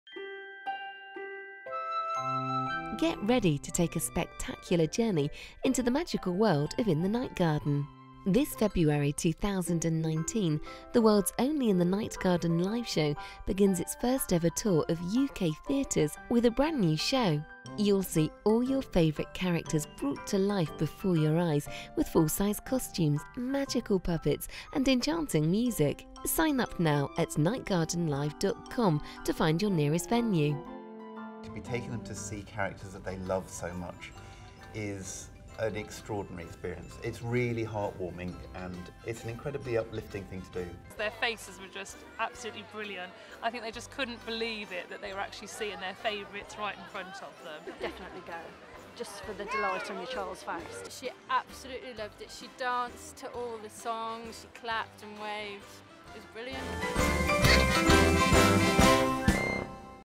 In The Night Garden Live at Hackney Empire
Igglepiggle, Upsy Daisy, Makka Pakka and friends are back in their brand new live show Igglepiggle’s Busy Day! Coming to Hackney Empire 1 & 2 Jun.